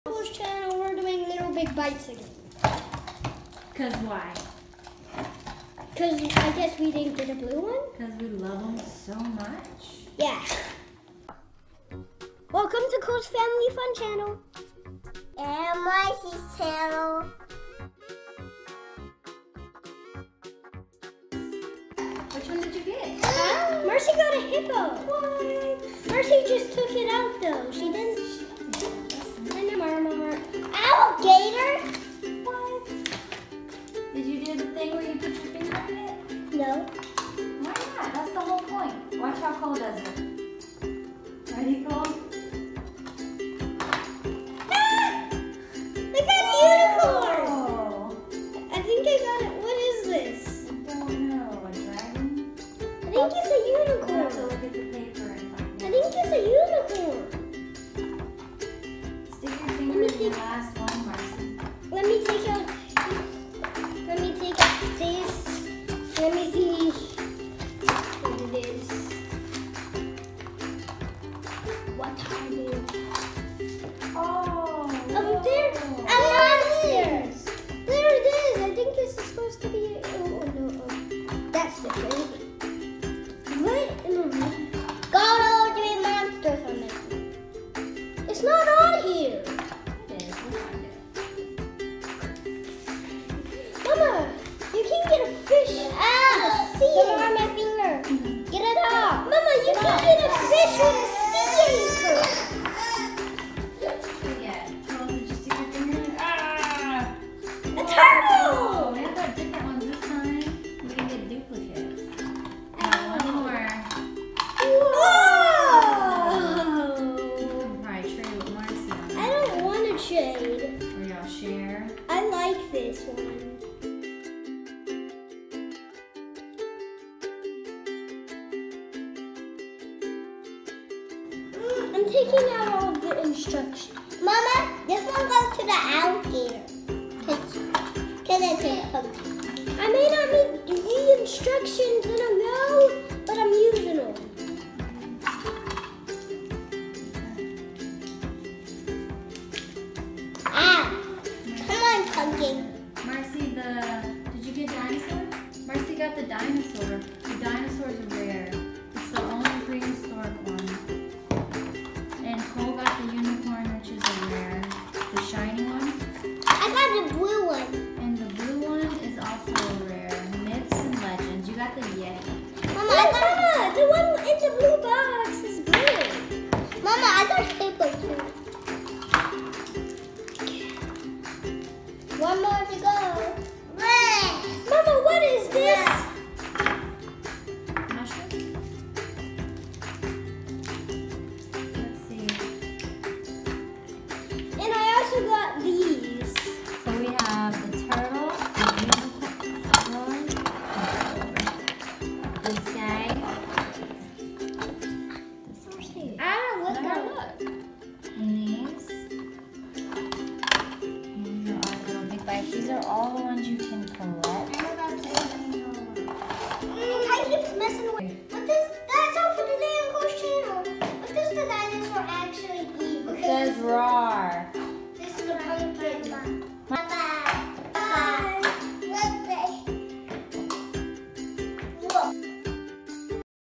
Little Big Bites by FurReal series 1 Opening !!!!
hey guys ! we got some more littlebigbites to open because we thought they were so much fun !
join us this time to see what littlebigbites we this time ! furreal toys,